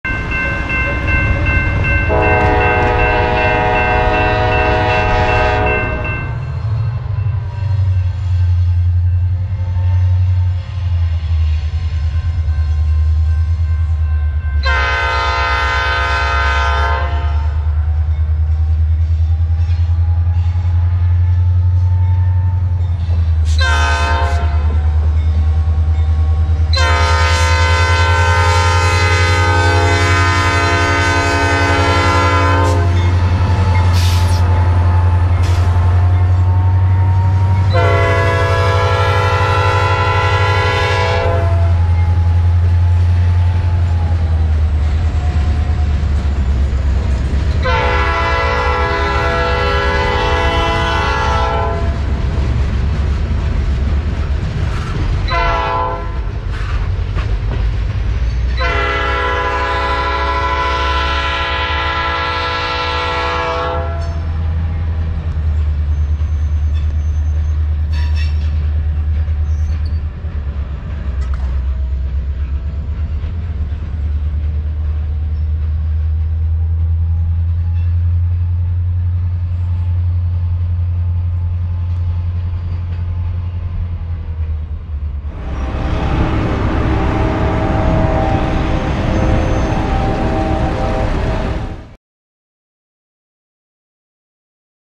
S01E125 A Slug of a Different Species (Norfolk Southern, Short Freight, GP40-2, Slug, ex-CR, ex-PC)
One minute and 34 seconds. That's all it will take you to click through the thumbnail and enjoy this entire video.  Did you know that some slugs now retain their cabs and can even be used as lead units on road freights? That's the whole idea behind Norfolk Southern's  RP-E4C slugs. NS 706, seen in this video pulling a short cut of covered hoppers, actually began life as a Penn Central GP38. Subscriptions are welcome.